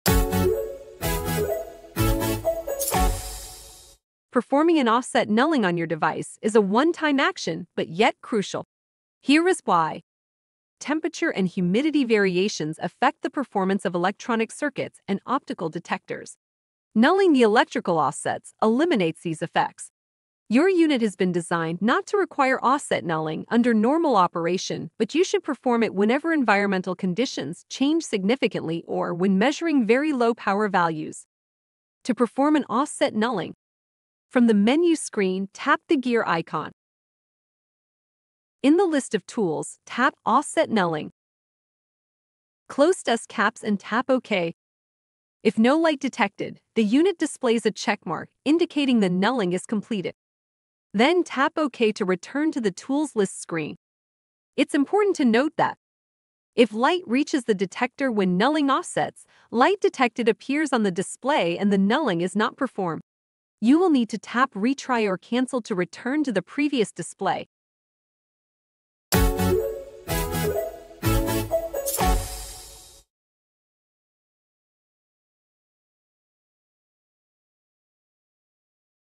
Perform an offset nulling on your EXFO PPM-350D | How-To
🔧 Mastering your PPM-350D? Don't overlook the importance of offset nulling! 🛠️ Learn why it's crucial to calibrate your device against temperature and humidity variations. Ensure optimal performance for precise measurements. Watch now and never miss a beat with your EXFO equipment!

TIMESTAMPS:
0:00 Intro
0:31 Perform Nulling